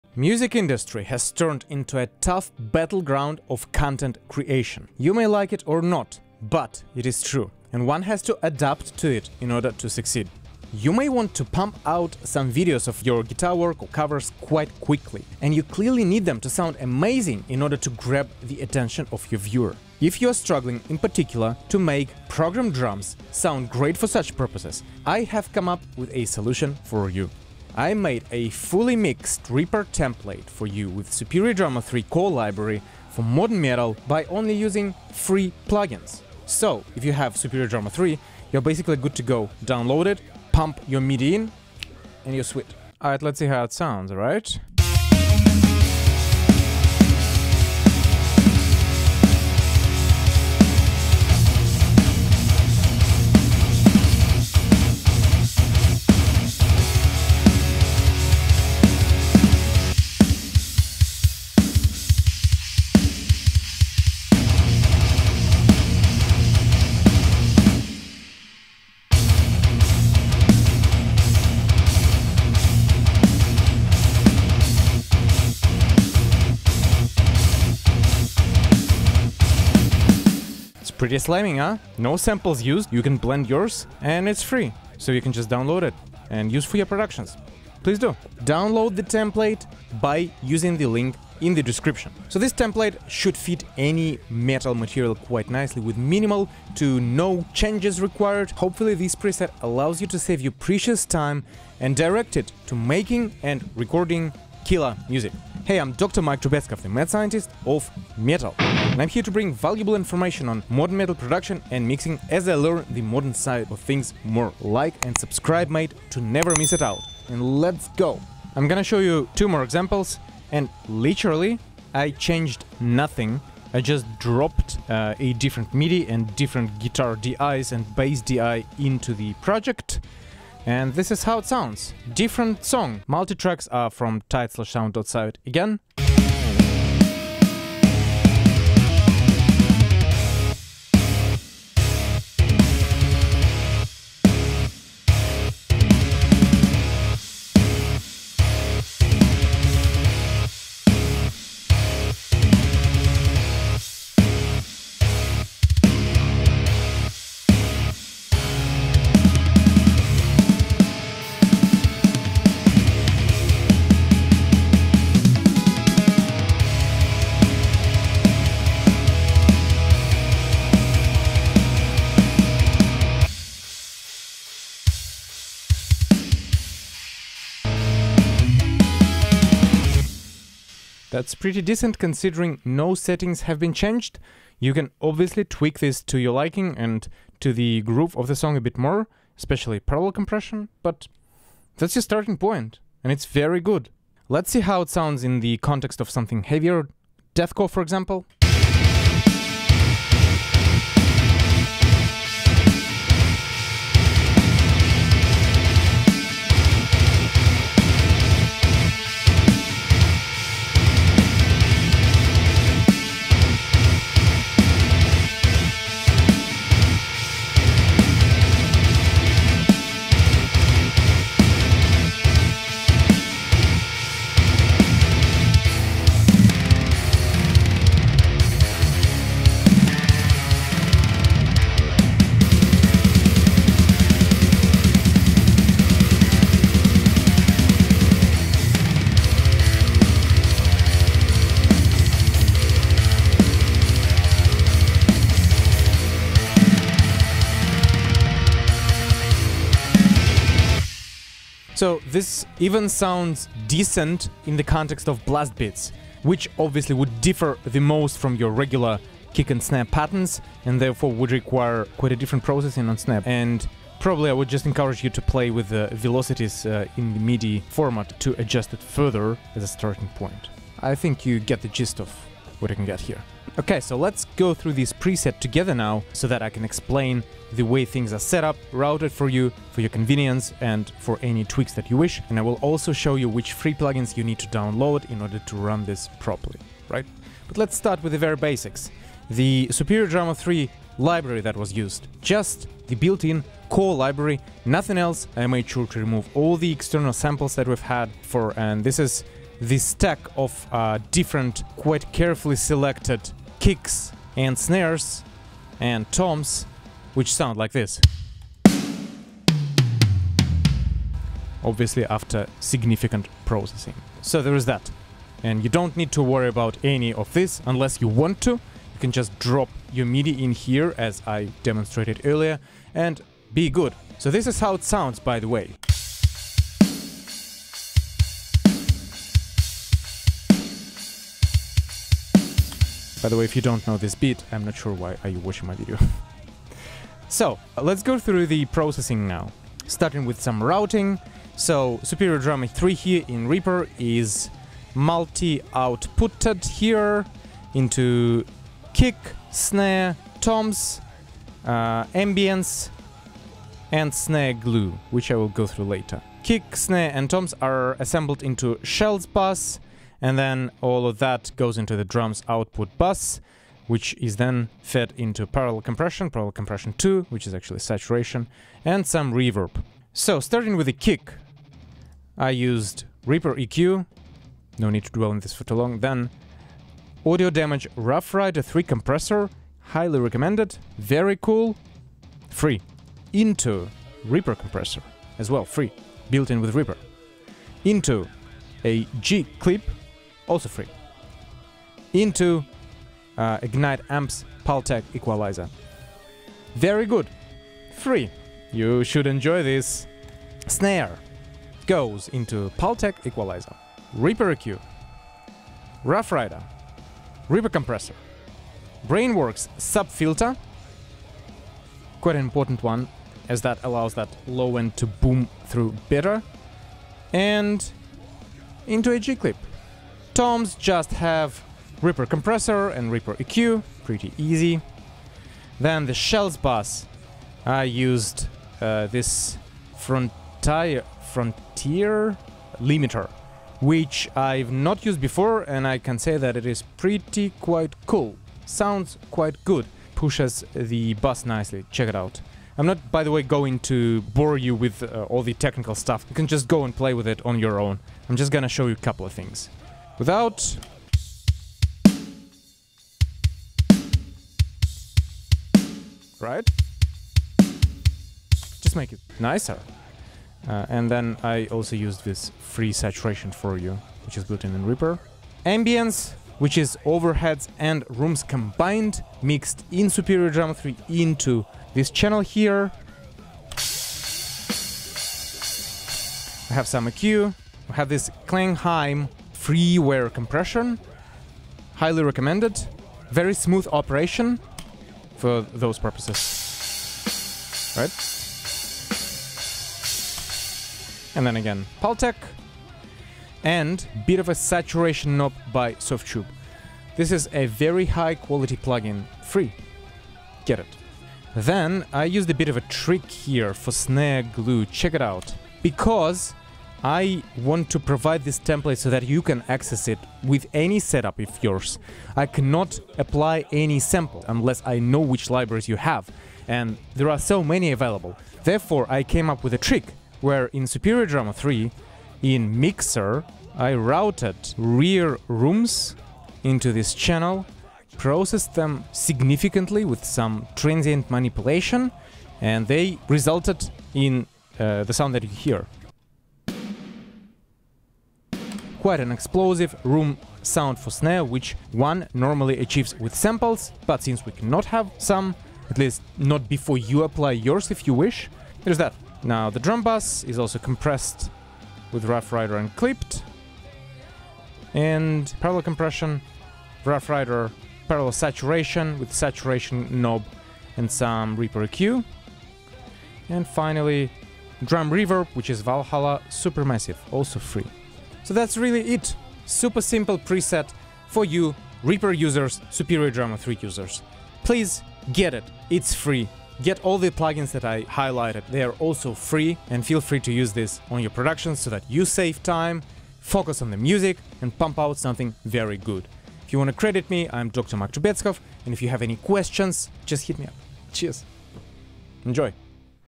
Unlock Epic Metalcore Drum Mixes in Seconds - Free Template for Reaper & Superior Drummer 3 Revealed
Free Mix-Ready Template for Modern Metal and Metalcore Drums in Reaper with Superior Drummer 3.

Use this template to make high-quality content faster!

0:00 Intro
0:45 Drum Sound Example
01:20 Download Free Reaper and Superior Drummer 3 Modern Drums Template
02:00 More Drum Sound Examples
02:15 Drum Sound Example 2
03:20 Drum Sound Example 3
04:33 Session Walkthrough
05:25 Drums in Solo Sound Example
05:39 Session Routing
06:09 Session Processing and Free Plugins
09:28 Conclusions